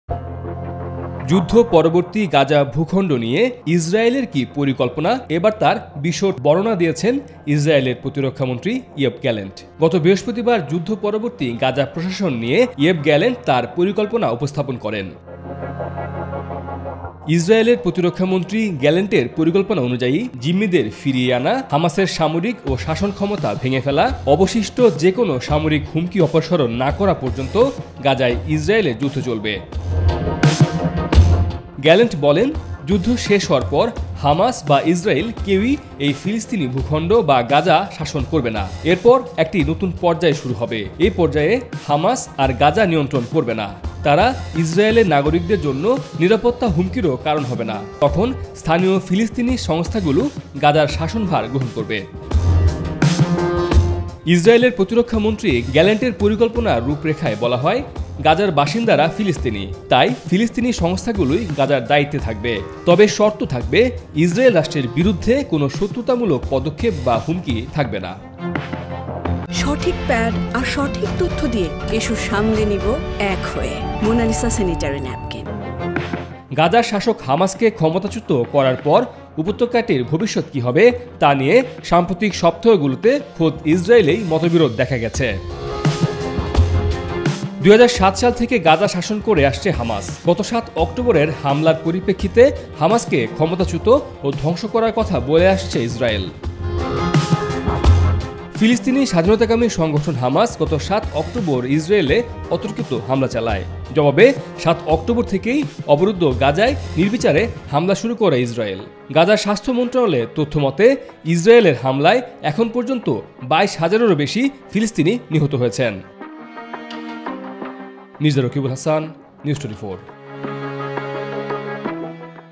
প্রকাশ পেল গাজা নিয়ে ইসরায়েলের পরিকল্পনা | Israel | Gaza | Yoav Gallant | News24
যু* দ্ধ–পরবর্তী গাজা ভূখন্ড নিয়ে ইসরায়েলের কি পরিকল্পনা এবার তার বিশদ বর্ণনা দিয়েছেন ইসরায়েলের প্রতিরক্ষামন্ত্রী ইয়োভ গ্যালান্ট। গত বৃহস্পতিবার যু* দ্ধ–পরবর্তী গাজা প্রশাসন নিয়ে ইয়োভ গ্যালান্ট তাঁর পরিকল্পনা উপস্থাপন করেন।

NEWS24 Team